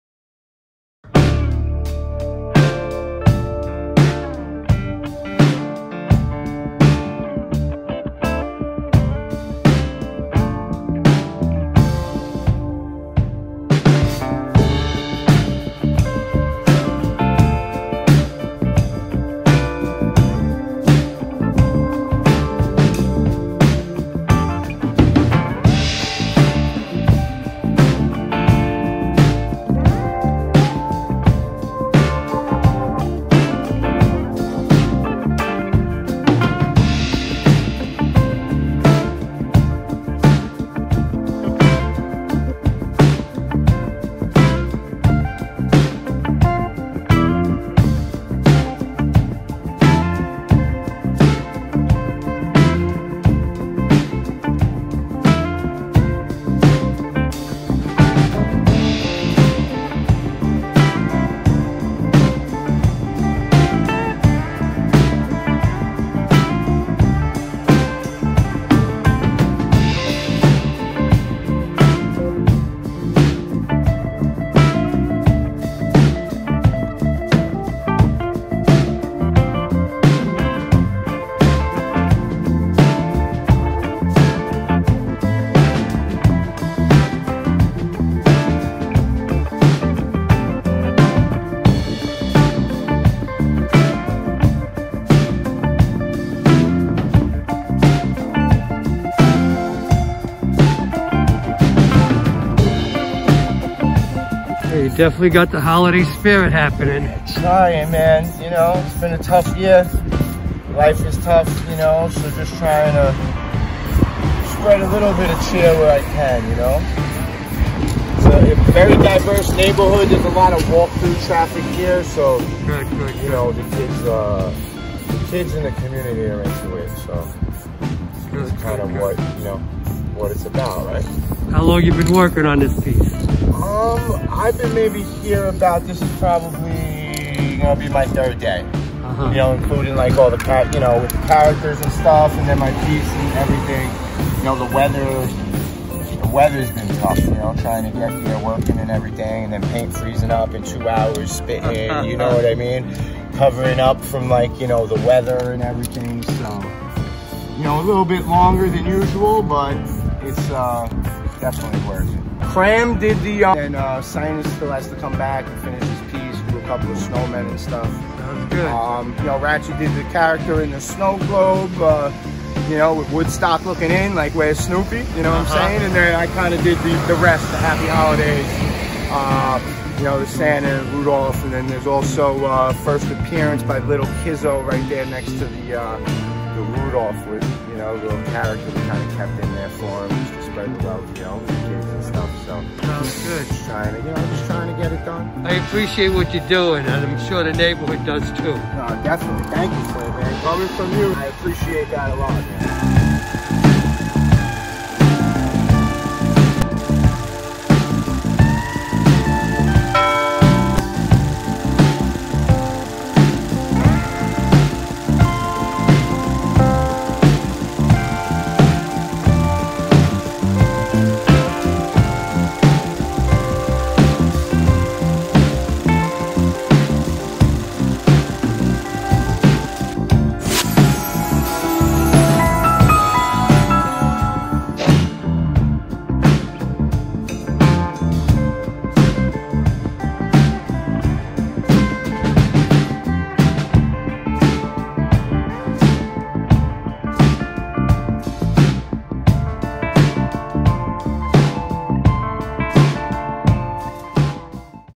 Meet the Artist- JohnExit
In another installment of the Meet the Artist series, here is a video about Staten Island-native John Exit.

Video by Flint Gennari

Edited by Chris Raimonto